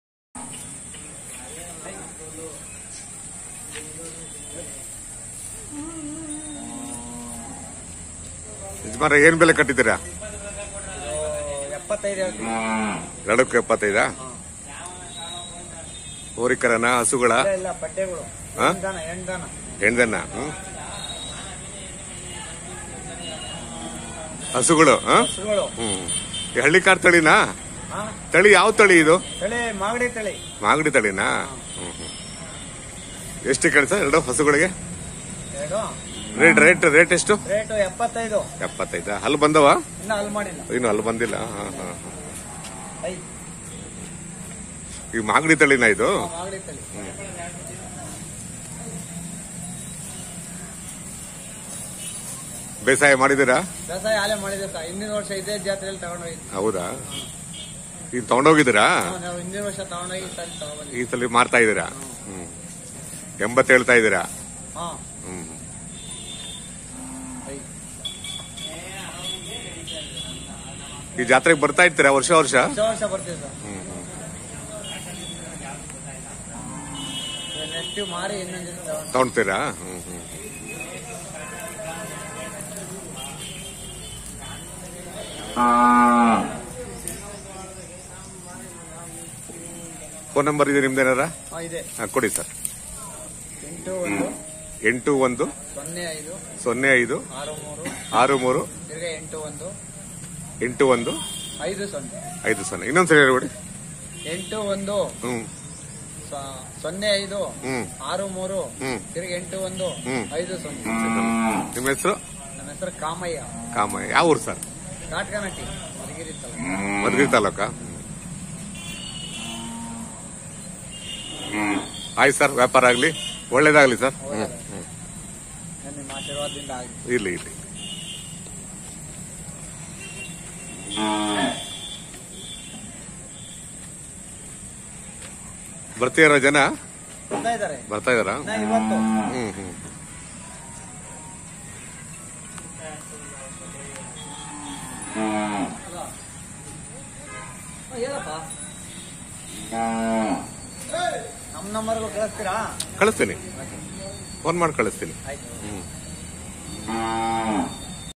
ಅದ್ದೂರಿ ಬಡವನ ಹಳ್ಳಿ ದನಗಳ ಜಾತ್ರೆ ಆರಂಭ ವಾಗಿದೆ
the historic beginning of badavanahalli cattle fair at madhugiri taluk
badavanahalli cattle fair 2023
ಕಾಮಯ್ಯ